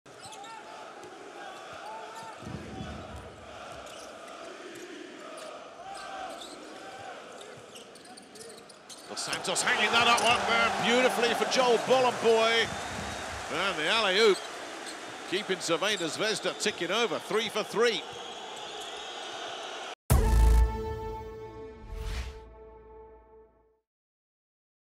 Magic Moment of the Night: Bolomboy's alley-oop dunk!
Joel Bolomboy gets the ball from Yago dos Santos and throws it down with authority!